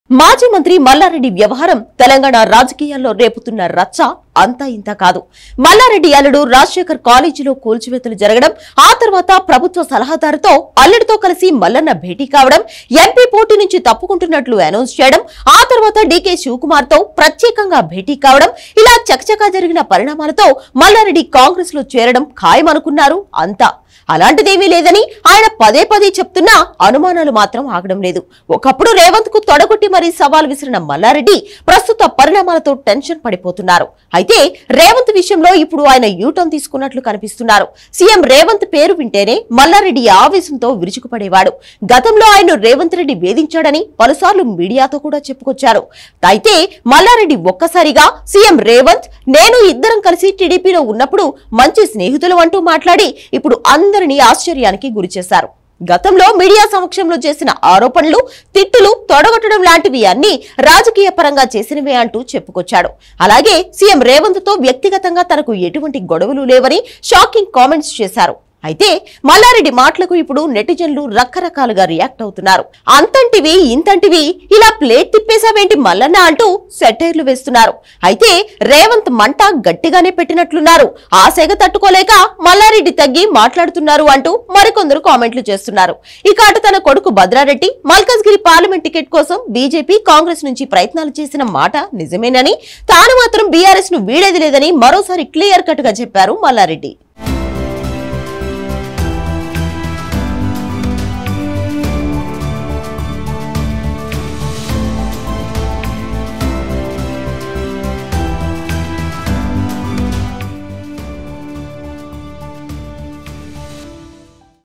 రేవంత్‌ నా దోస్త్‌ ఆ విషయం ఎప్పుడో చెప్పా || Malla Reddy About CM Revanth Reddy | Dial News
రేవంత్‌ నా దోస్త్‌ ఆ విషయం ఎప్పుడో చెప్పా..ఏంది మల్లన్న..ఇలా ప్లేట్ తిప్పేశావ్‌ || Malla Reddy About CM Revanth Reddy | Dial News